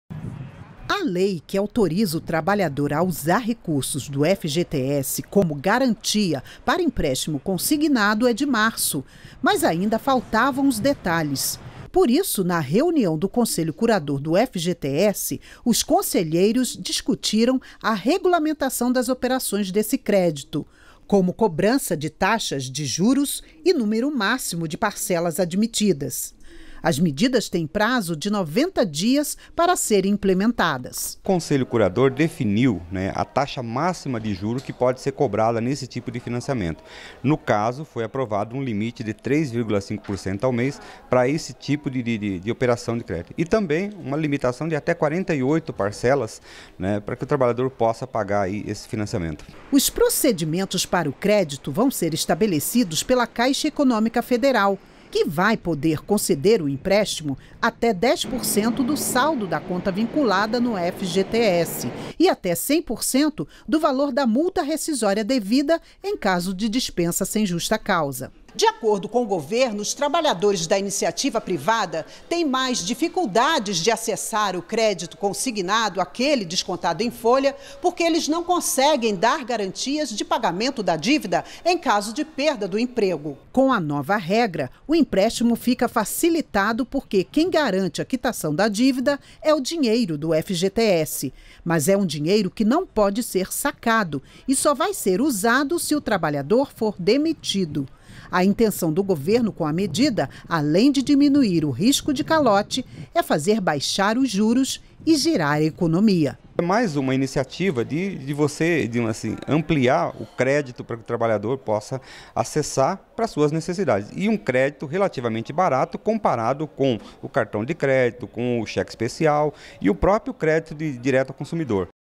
Regulamentado o uso do FGTS como garantia de empréstimos consignados
REPÓRTER NBR - 06.12.16: O Conselho Curador do FGTS definiu as regras para operações que permitem ao trabalhador utilizar o benefício como garantia de empréstimos consignados.